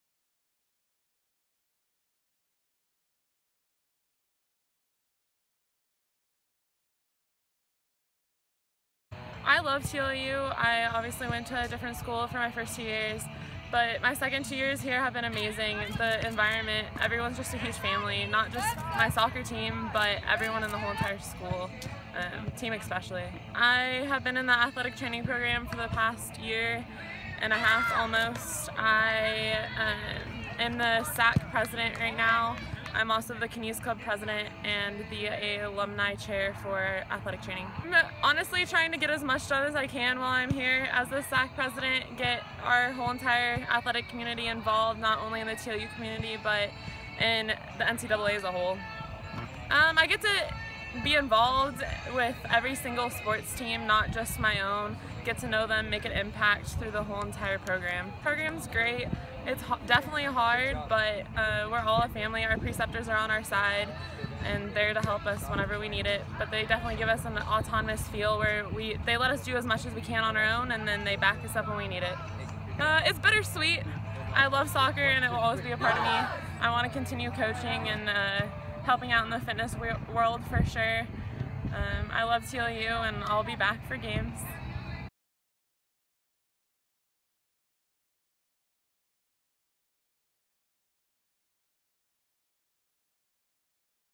Character & Community (Week 2) - Selena Wicklund, Texas Lutheran University
Selena Wicklund of Texas Lutheran University​ is the Character & Community Female Student-Athlete of the Week!